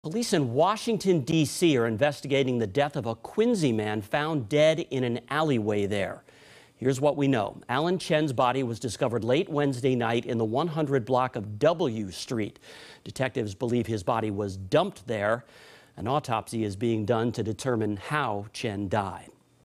Quincy man found dead in Washington, DC alley
An investigation is underway after a Quincy man was found dead in Washington, DC.